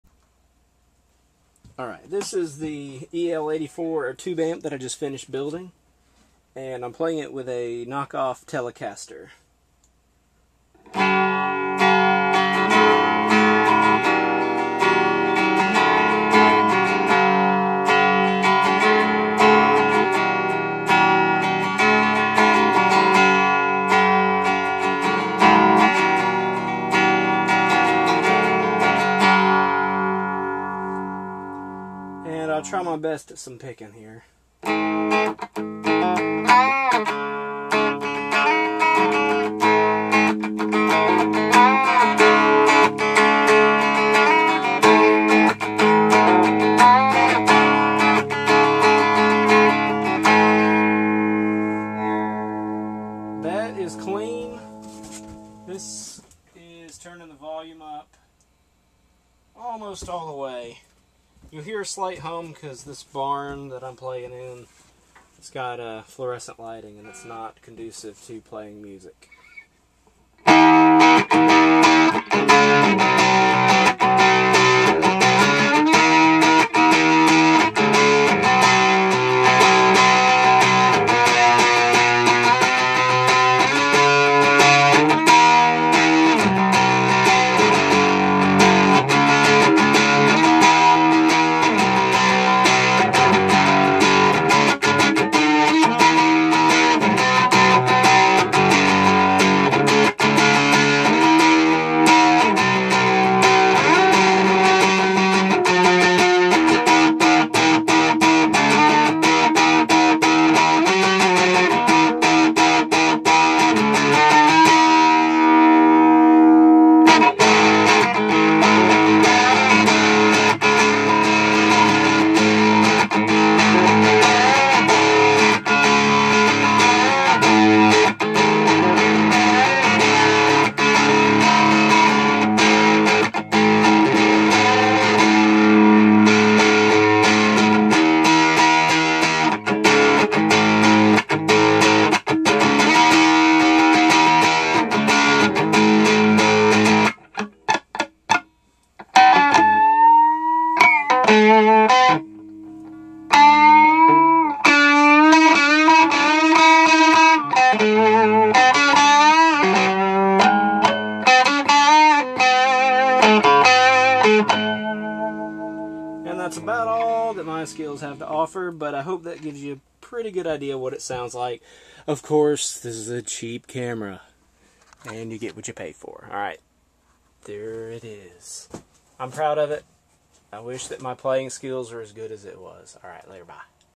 EL84 Tube Amp build completed. Schematic @ tech-tut.com
This is the second tube amp build of mine. This amp uses a 12ax7 preamp and an EL84 power amp. According to the EL84 data sheet, this amp is a 6 watt output.

The schematic and some info about the build can be found at my
This is the page with all of my tube amp articles/blogs.